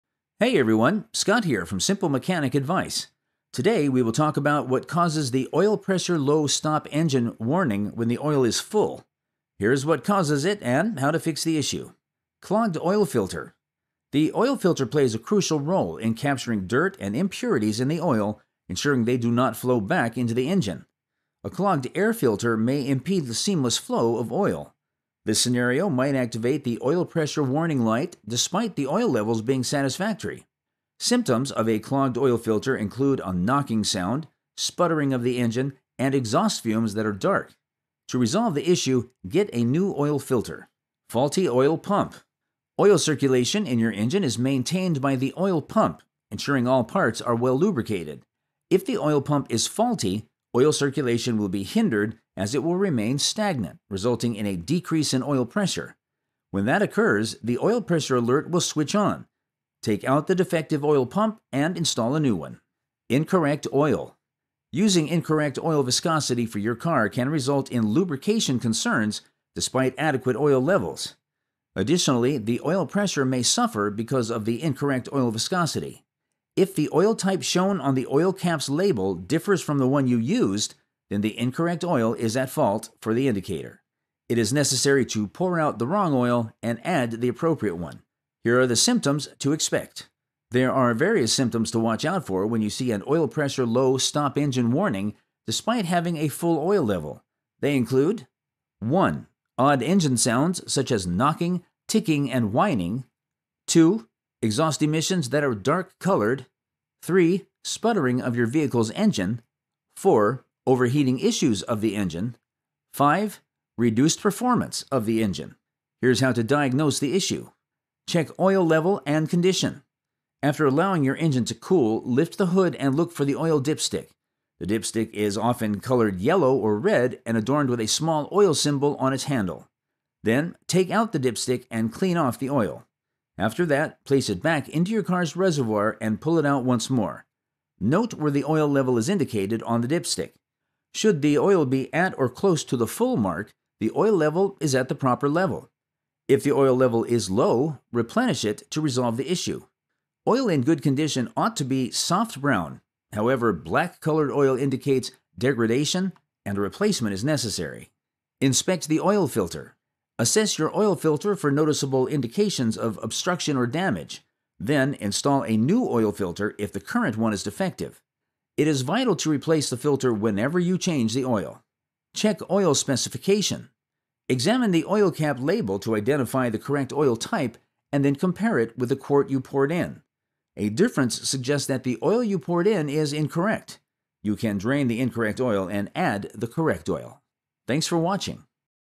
‘Oil Pressure Low Stop Engine’ Warning But Oil Is Full: Causes Symptoms And Diagnosis
‘Oil Pressure Low Stop Engine’ Warning But Oil Is Full: Causes, Symptoms And Diagnosis. In this video tutorial I will show you how to fix "Oil Pressure Low" warning with full oil.

Here is what causes it and how to fix the issue:

Clogged oil filter
The oil filter plays a crucial role in capturing dirt and impurities in the oil, ensuring they do not flow back into the engine.
To resolve the issue, get a new oil filter.

Faulty oil pump.
If the oil pump is faulty, oil circulation will be hindered as it will remain stagnant, resulting in a decrease in oil pressure. When that occurs, the oil pressure alert will switch on. Take out the defective oil pump and install a new one.

Incorrect oil
Using incorrect oil viscosity for your car can result in lubrication concerns, despite adequate oil levels.
It is necessary to pour out the wrong oil and add the appropriate one.

Here are the symptoms to expect:
There are various symptoms to watch for when you see an Oil Pressure Low Stop Engine warning despite having a full oil level. They include:
1. Odd engine sounds, such as knocking, ticking, and whining.
2. Exhaust emissions that are dark-colored.
3. Sputtering of your vehicle’s engine.
4. Overheating issues of the engine.
5. Reduced performance of the engine.

Here’s how to diagnose the issue:
Check oil level and condition.
• After allowing your engine to cool, lift the hood and look for the oil dipstick. The dipstick is often colored yellow or red and adorned with a small oil symbol on its handle.
• Then, take out the dipstick and clean off the oil.
• After that, place it back into your car’s reservoir and pull it out once more.
• Note where the oil level is indicated on the dipstick.
• Should the oil be at or close to the full mark, the oil level is at the proper level.
• If the oil level is low, replenish it to resolve the issue.
• Oil in good condition ought to be soft brown. However, black-colored oil indicates degradation and a replacement is necessary.

Inspect the oil filter
• Assess your oil filter for noticeable indications of obstruction or damage.
• Then, install a new oil filter if the current one is defective.
• It is vital to replace the filter whenever you change the oil.

Check oil specification
Examine the oil cap label to identify the correct oil type, and then compare it with the quart you poured in. A difference suggests that the oil you poured in is incorrect. You can drain the incorrect oil and add the correct oil.